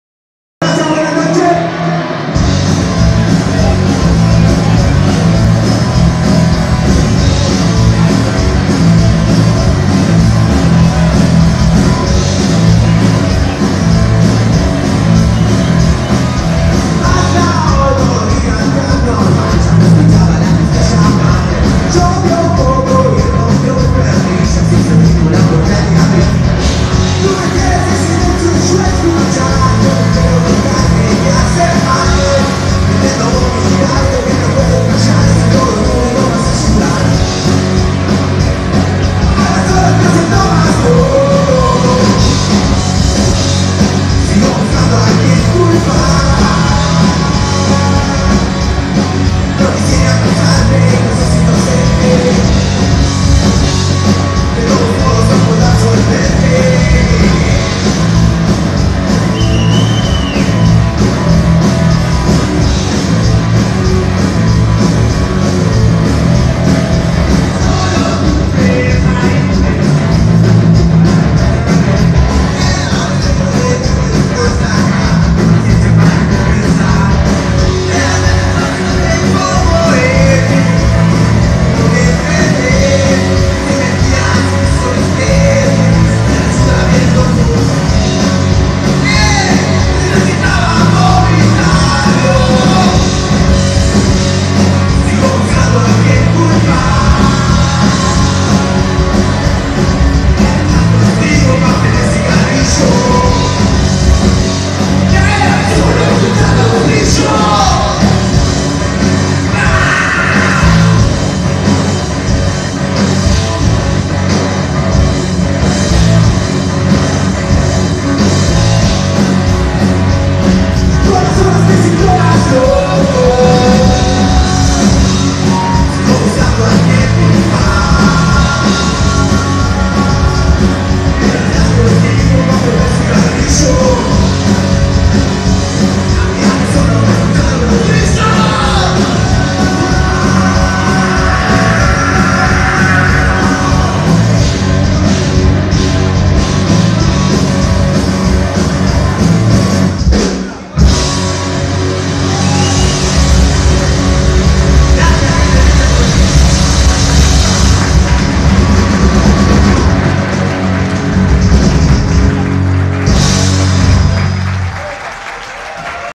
Runa Rock (Santa Fe)
destacado recital de la joven banda de rock RUNA en un bar de la capital provincial